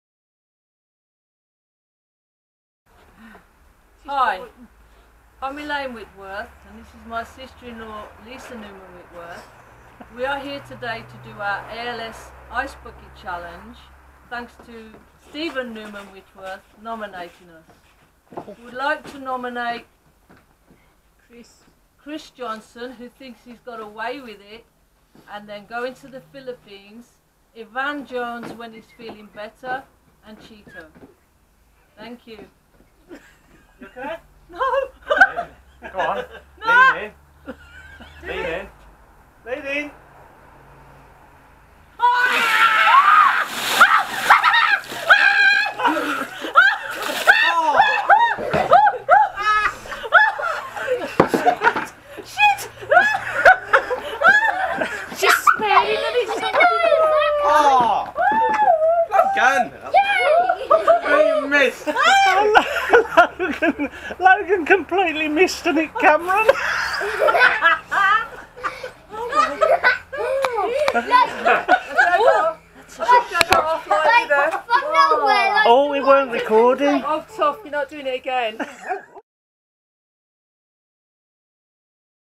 Lisa and Elaines A.L.S Ice Bucket Challenge
i nominated my wife and my sister also logan [other video]  to do the als ice bucket challenge and fair play to them they did it this was one of the funniest ones i have seen yet